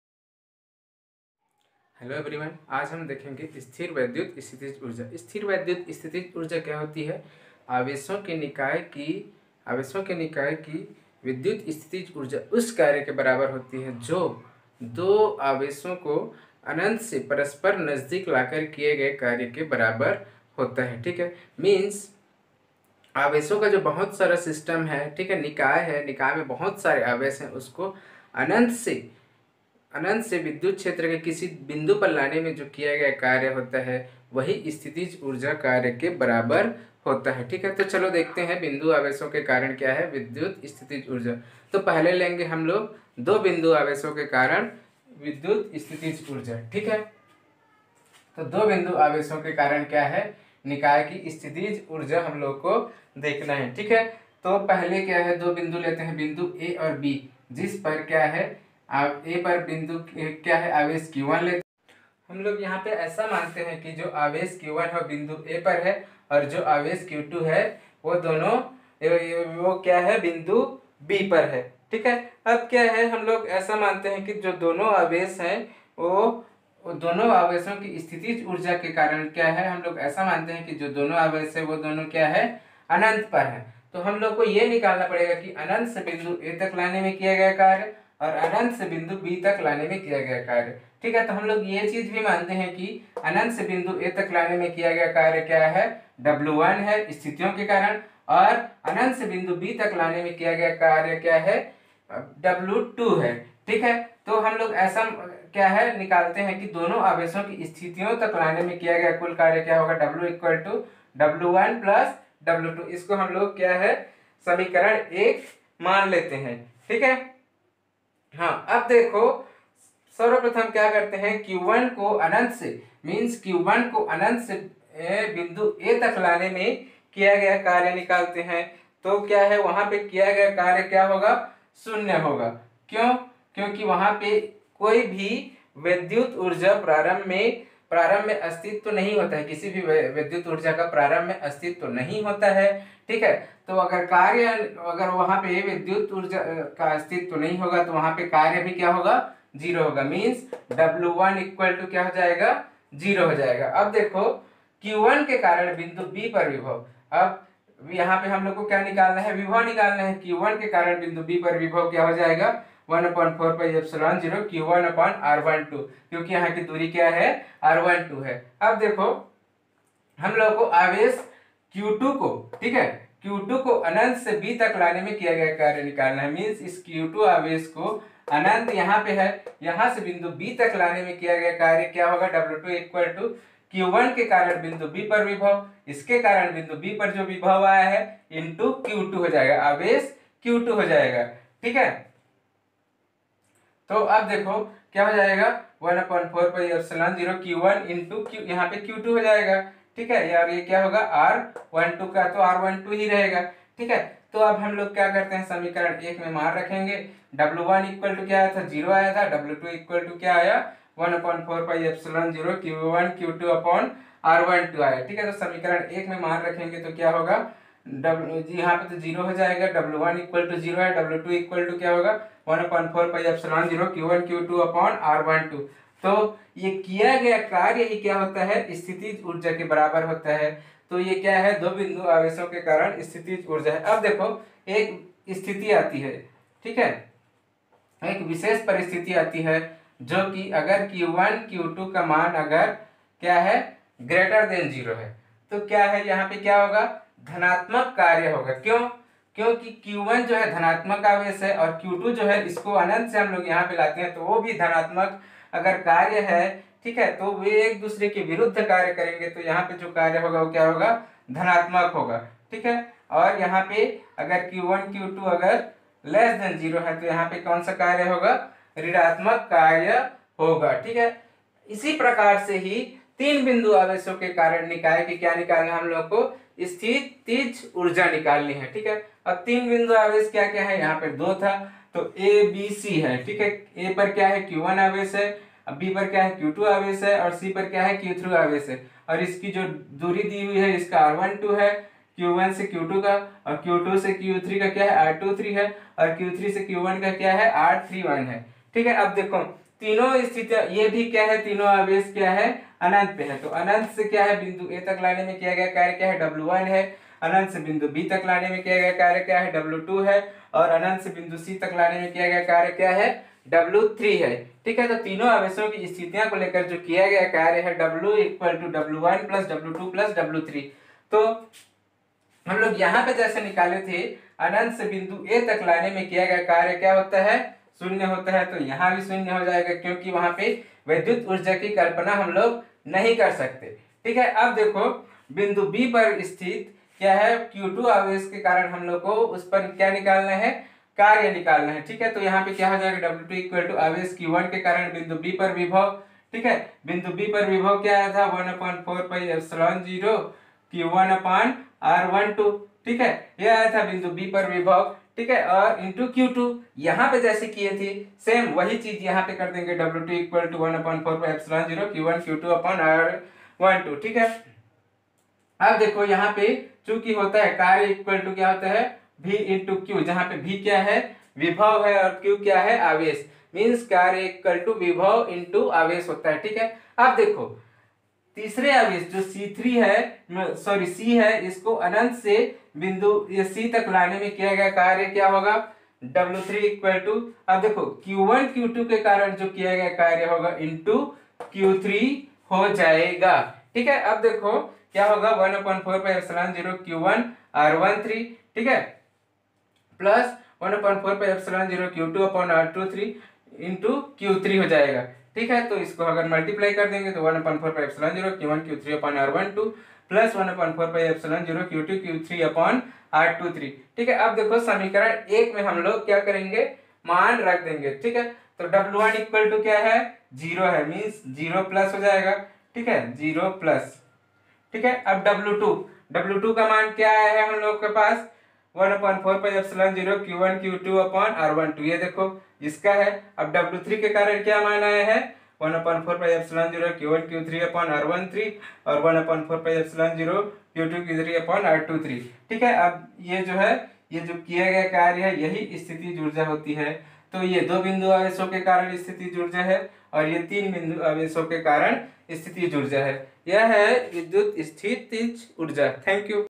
Physics Class 12 (NCERT)|| स्थिर वैद्युत स्थितिज ऊर्जा || Electrostatic Potential Energy
स्थिर वैद्युत स्थितिज ऊर्जा :- इसमें हम देखेंगे की दो बिंदु और तीन बिंदु आवेशों के कारण वैद्युत विभव क्या आता है और िस्थितिज ऊर्जा क्या होता है इसके बारे में भी देखेंगे